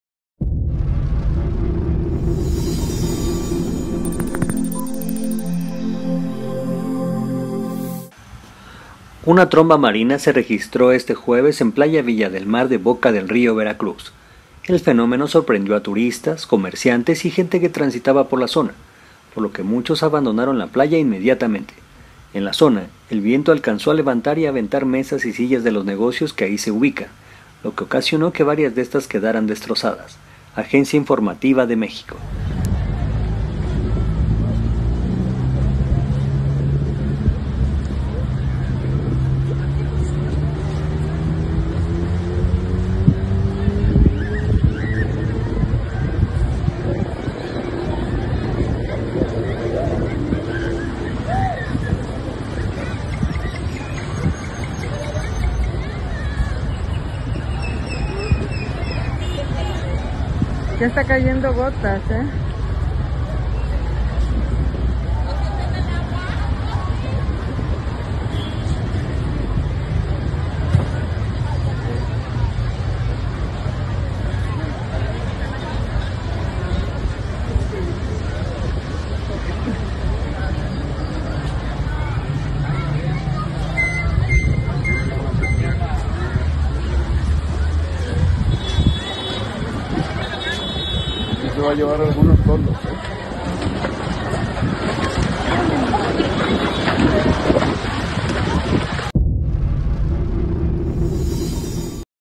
Tromba Marina y posterior tornado azota las costas de Veracruz, México.
El fenómeno sorprendió a turistas, comerciantes y gente que transitaba por la zona, por lo que muchos abandonaron la playa inmediatamente.